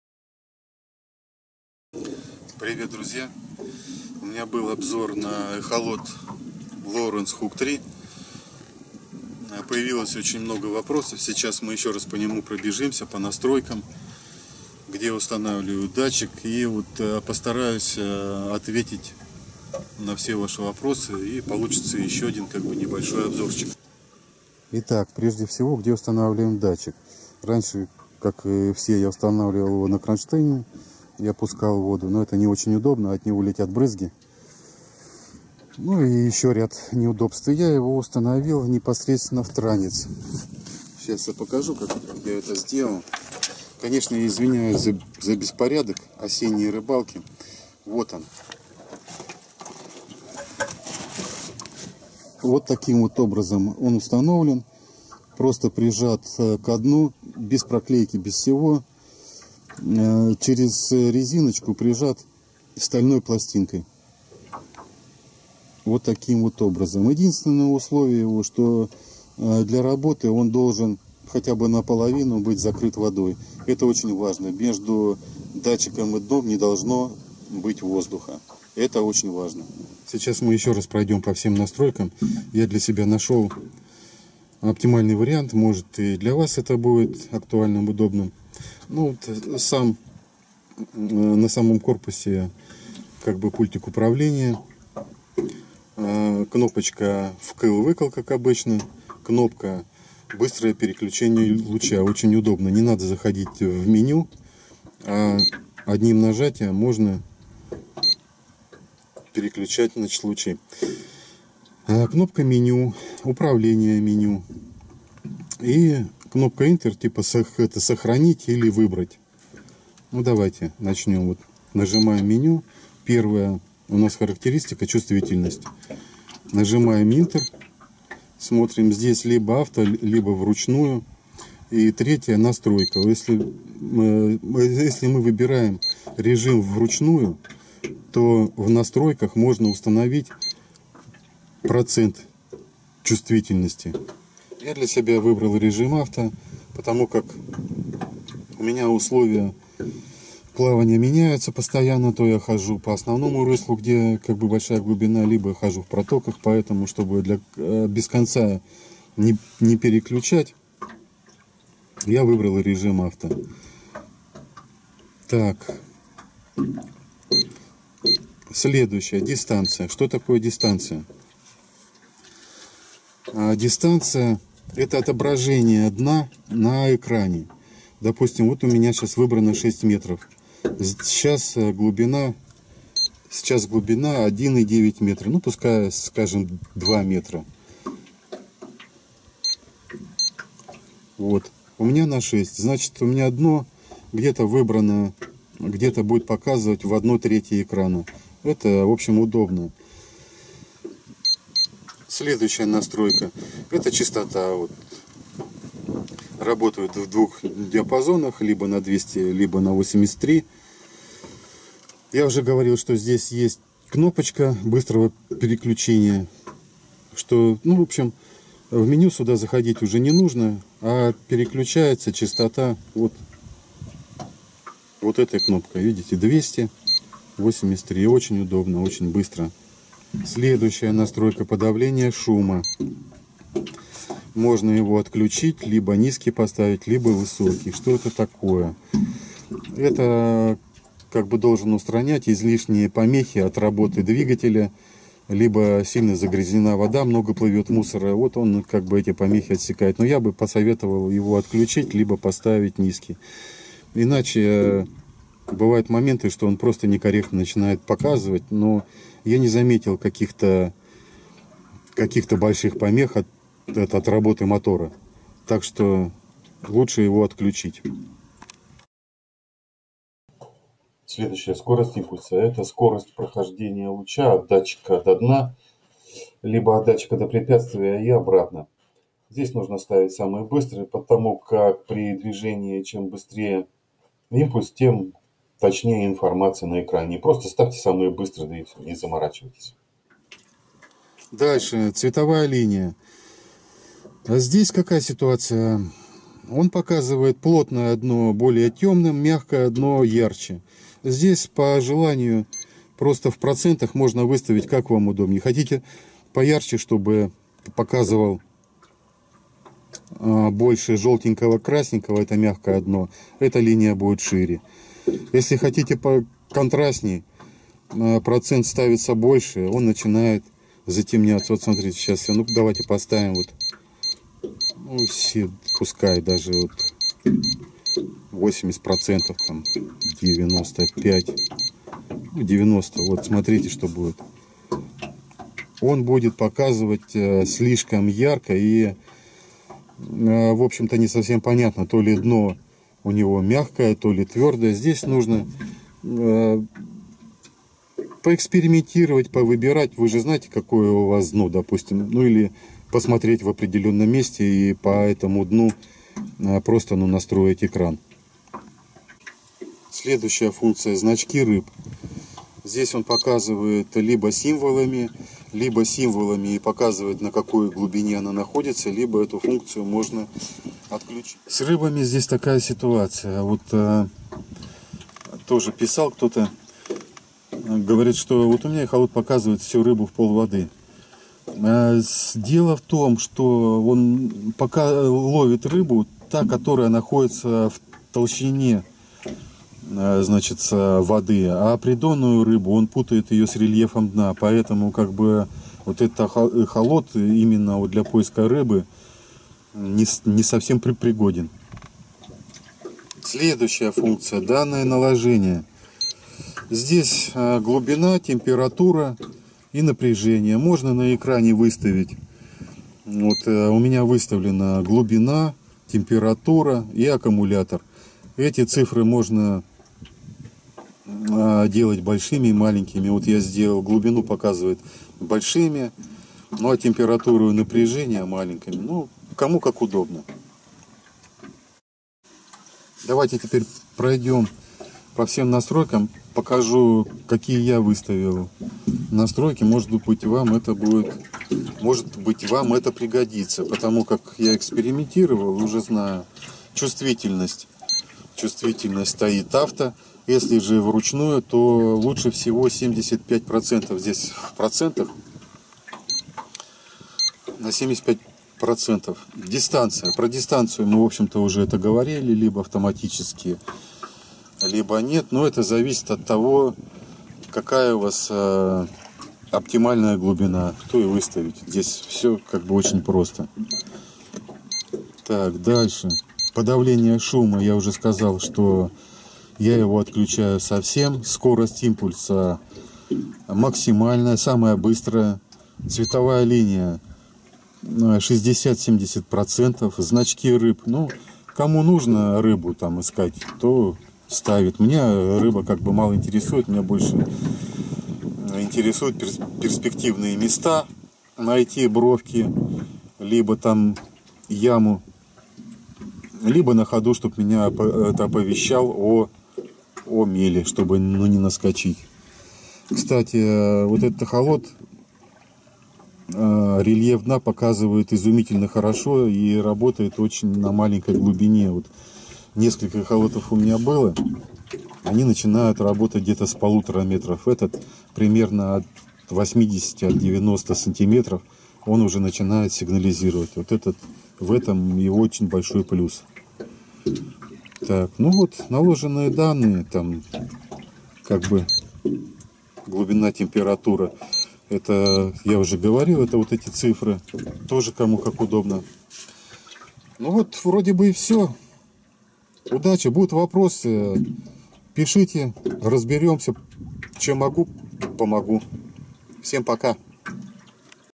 Эхолот Lowrance Hook 3x лучшее приобретение для удачной рыбалки
описание настроек Эхолота Lowrance Hook 3x